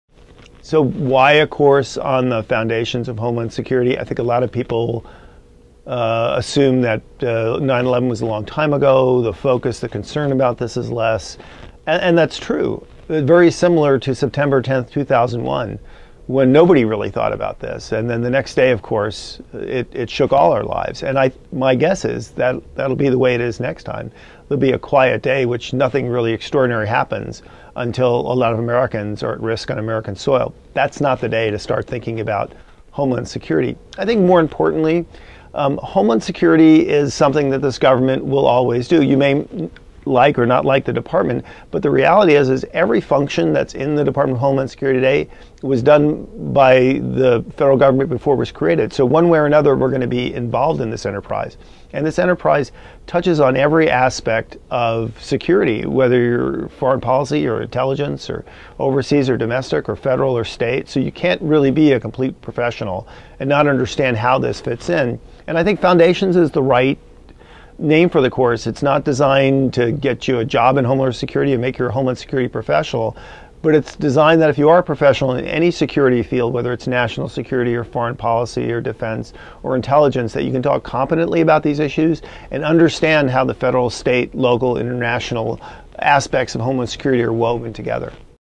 Foundations of Homeland Security (IWP 664)
Dr. James Jay Carafano discusses his course on Foundations of Homeland Security (IWP 664).

Dr. Carafano is Vice President of Foreign and Defense Policy Studies and Director of The Kathryn and Shelby Cullom Davis Institute for International Studies at The Heritage Foundation.